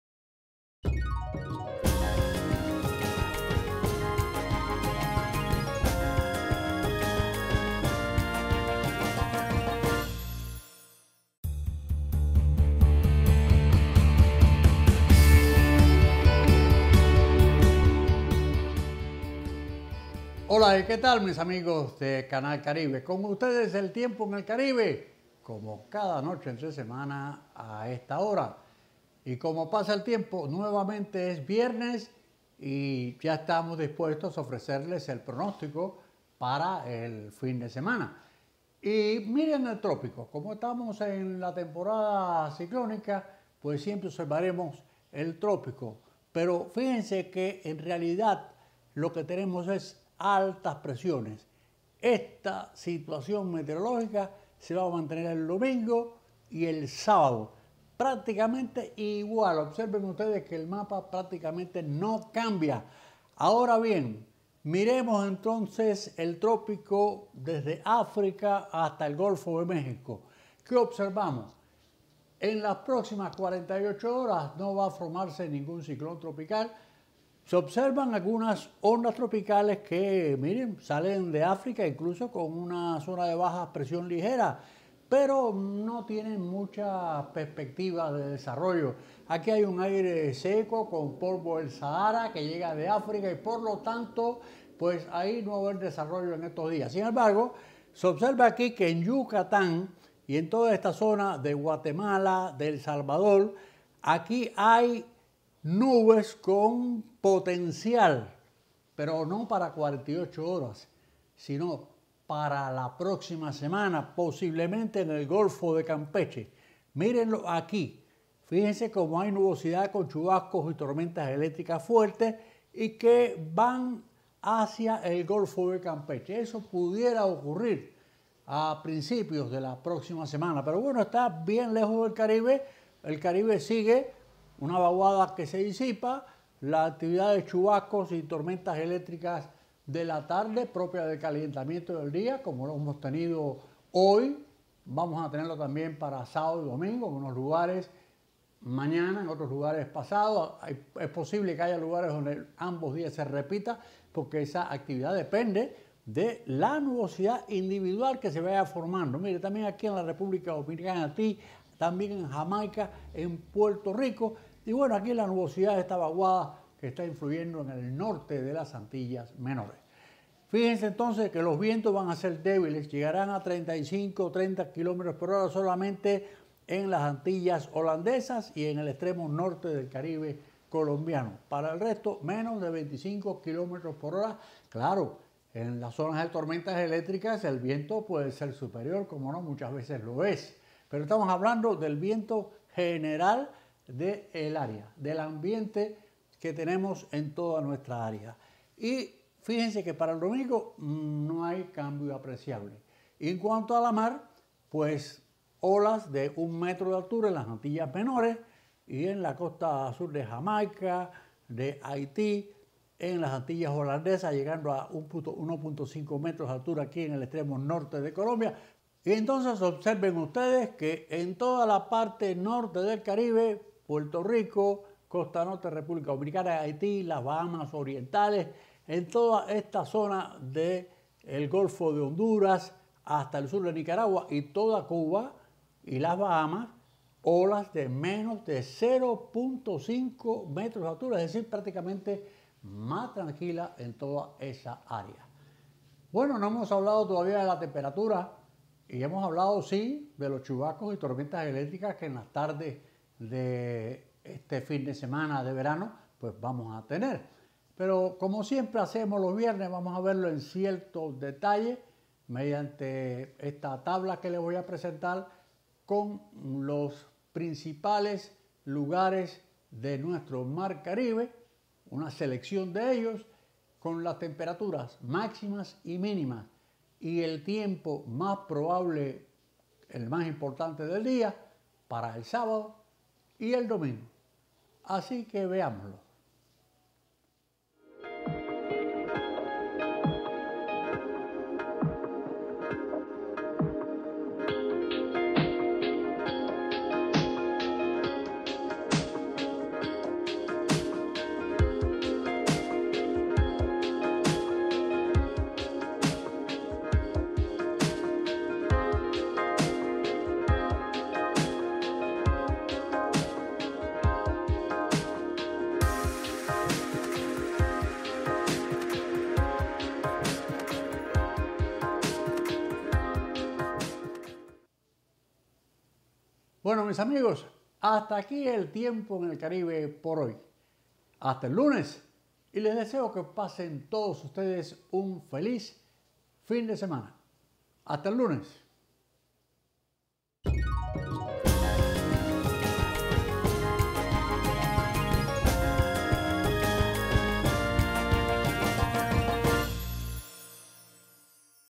El Tiempo en el Caribe | Válido 12 y 13 de junio de 2021 - Pronóstico Dr. José Rubiera desde Cuba
El DrRubiera comparte pormenores del pronóstico del tiempo para este fin de semana (12 y 13 de junio de 2021).